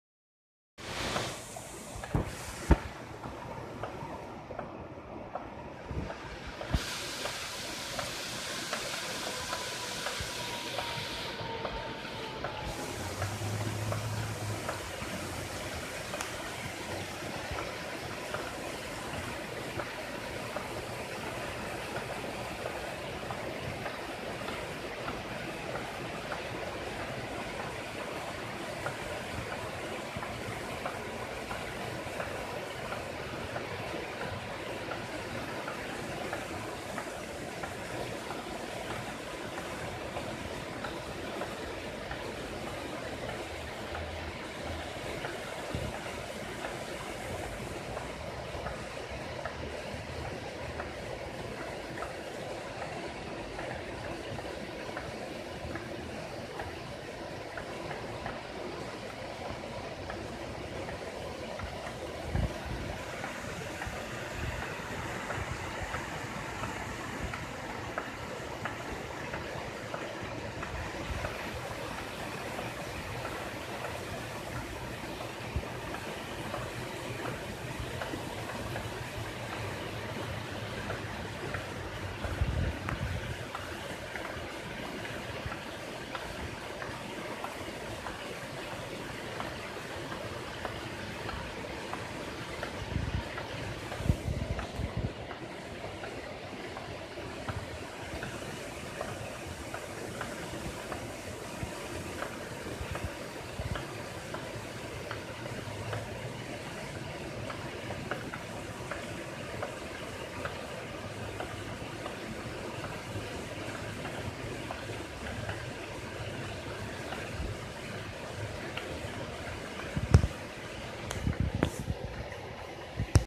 Another rainy day
I refer to the little stream as a raging river when the water rises.